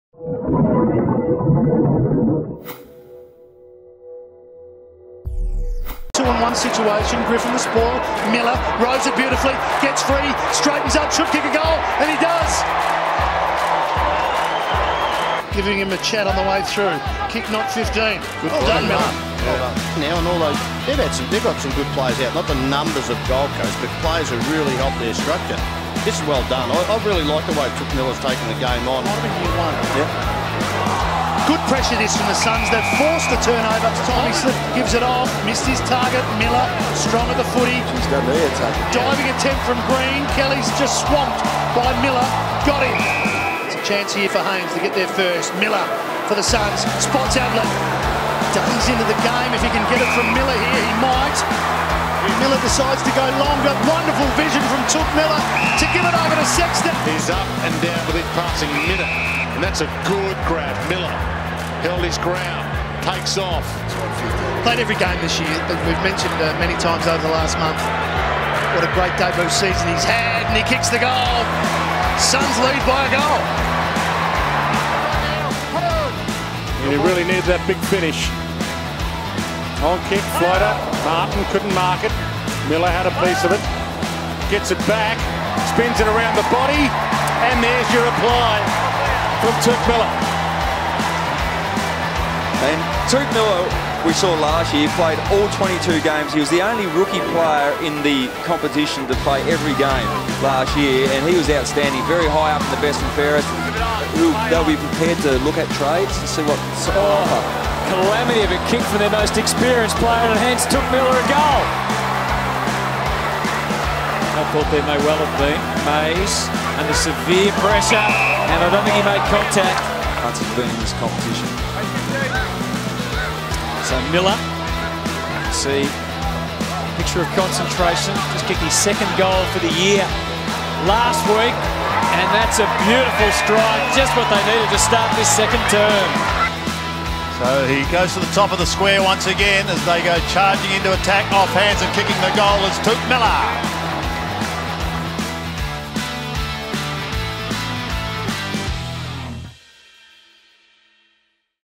Highlights: Touk Miller
Some of Touk Miller's best moments in the red and gold.